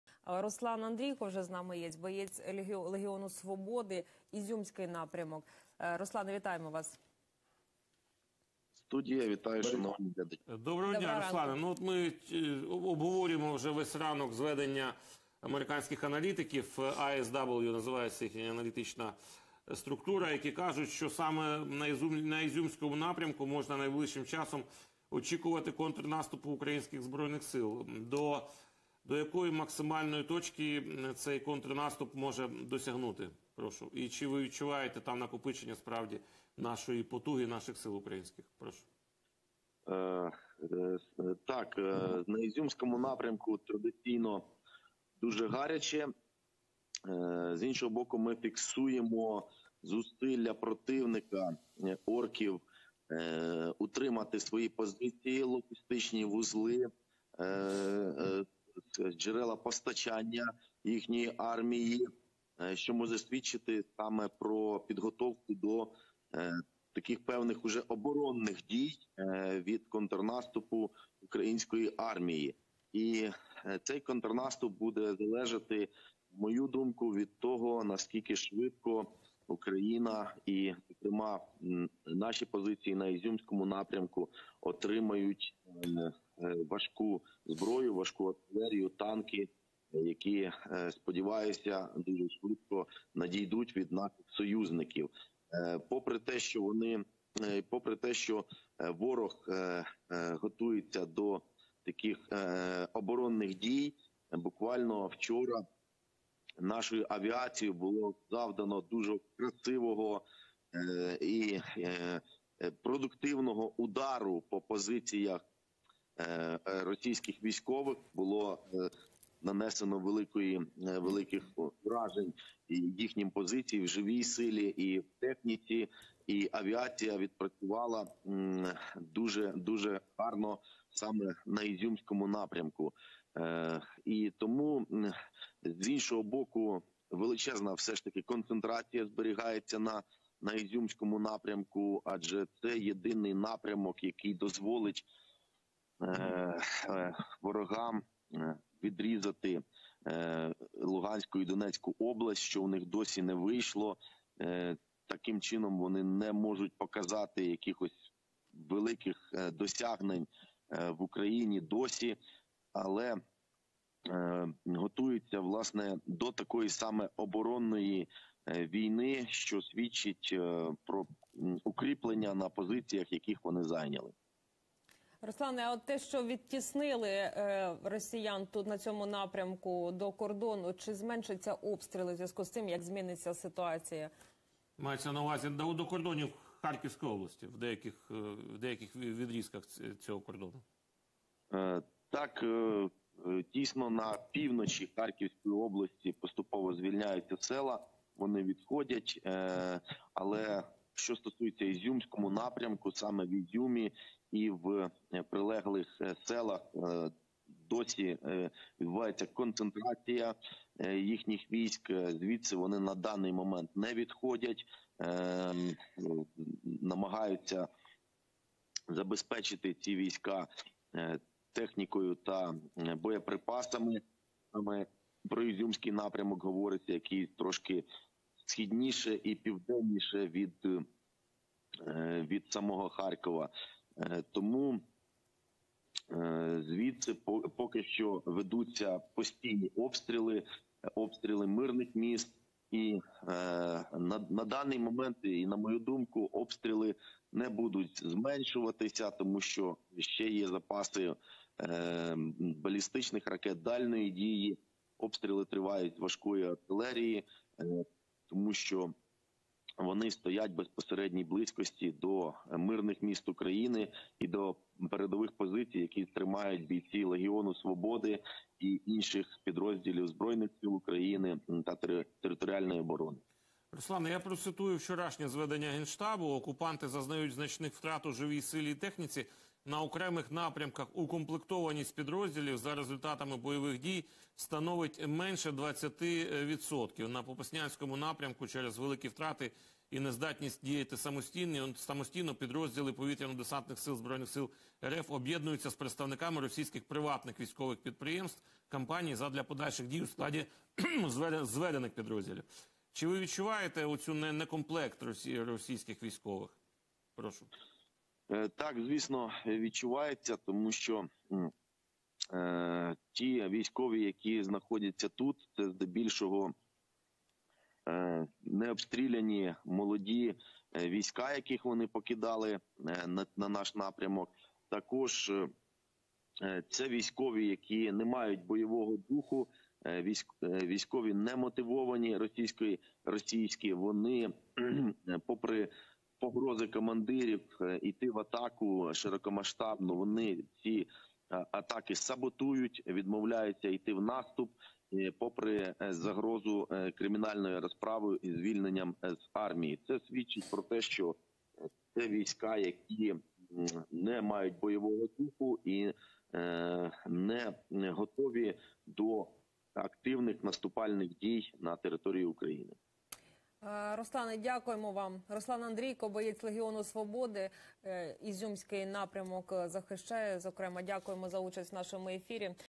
Ізюмський напрямок: Руслан Андрійко про ситуацію на фронті вранці 16 травня / Легіон Свободи
Руслан Андрійко, боєць добровольчого батальйону "Карпатська Січ" (Легіон Свободи) про ситуацію на Ізюмському напрямку станом на ранок 16 травня. Етер телеканалу "Еспресо".